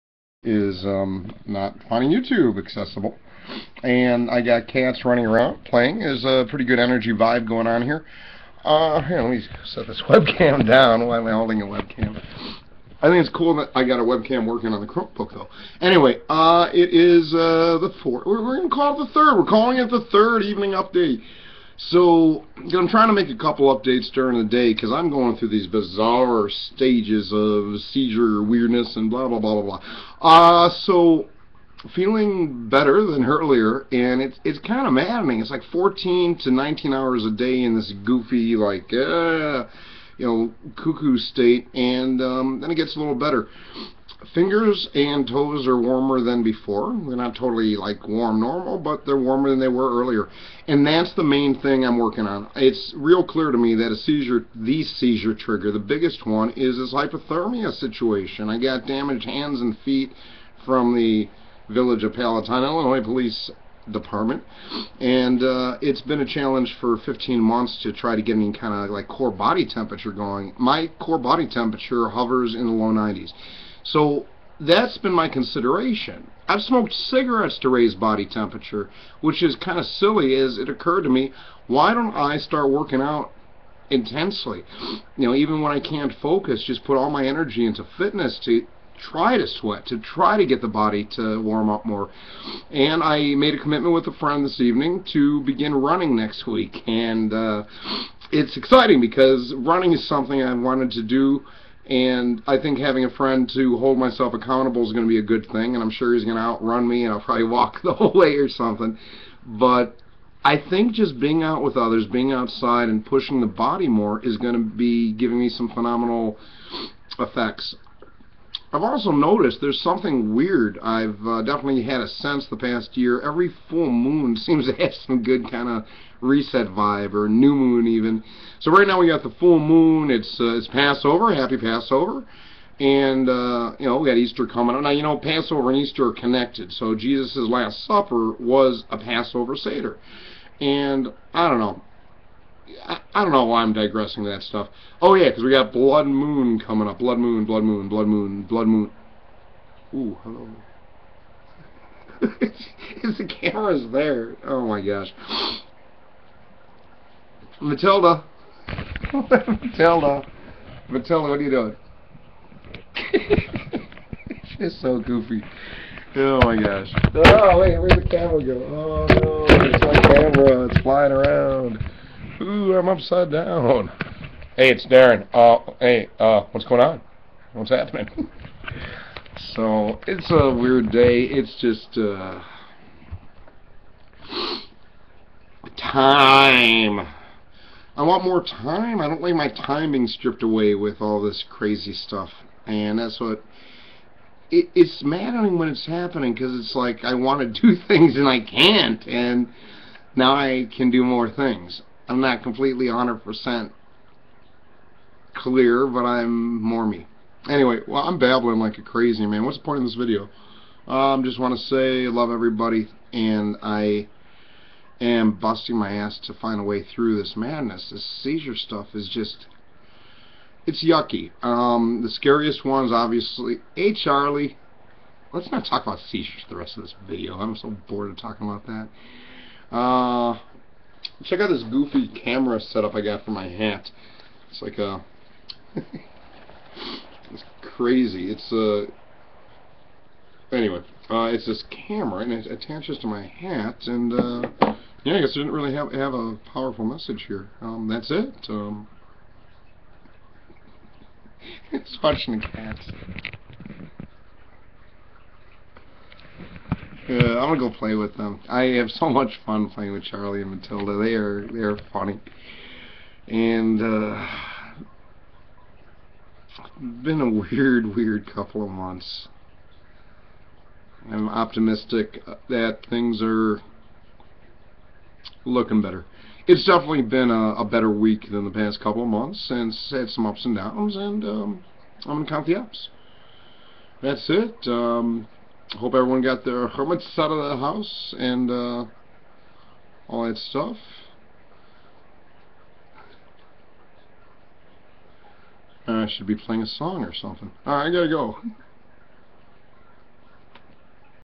040315 - Darren's Evening Update - Just Goofing Around at This Point
OMG what a weird day. I'm just babbling and laughing at cats and really don't have much of a message here. Have a great night! Darren